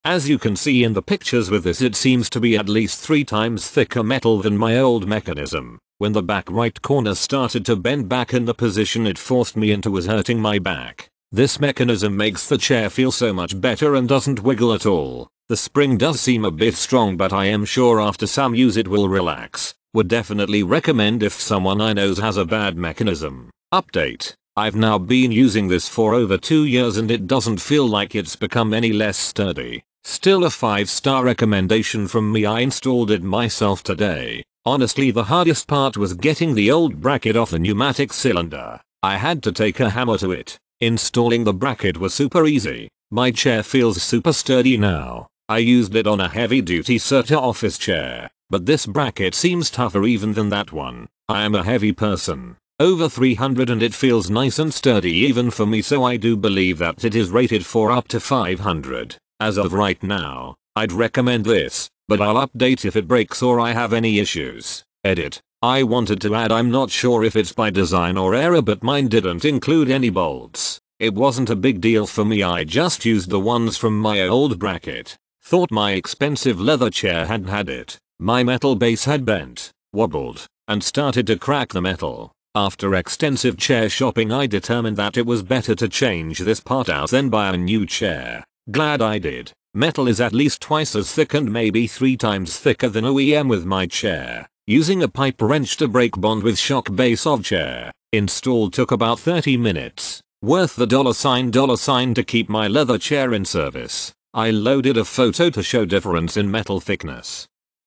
Heavy Duty Replacement Office Chair Swivel Tilt Control Seat Mechanism w// 6" x review
Heavy Duty Replacement Office Chair Swivel Tilt Control Seat Mechanism w// 6" x 10.2" Mounting Holes - S2979-HD

Main Features:
- Heavy duty replacement office chair tilt mechanism with 10.2" long x 6" wide screw mounting hole pattern
- Heavy duty - rated up to 500 lbs.
- Single lever for gas cylinder height activation - Accepts standard tapered gas lift cylinders
- Tilt control - push lever in to lock tilt, pull out to unlock
- Twist hand wheel to tighten or loosen (adjust) backward tilt tension